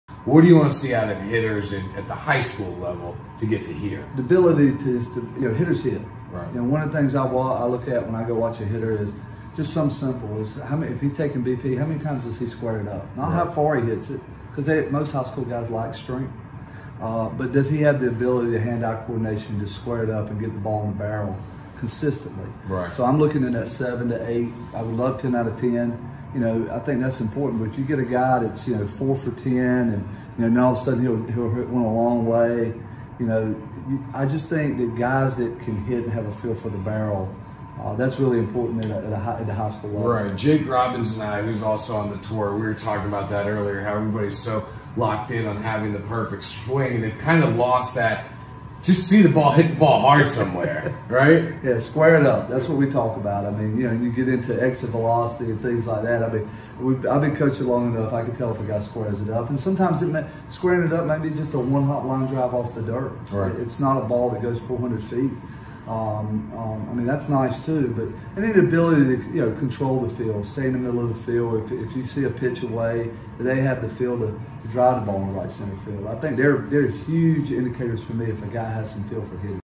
Evaluating Hitters // @JRudd_Scout // 2020 College Tour
In a clip from the 2020 College Tour, host Josh Rudd (@JRudd_Scout on Twitter and Instagram) asks UNC Greensboro head coach Billy Godwin to explain what he looks for in a high school hitter.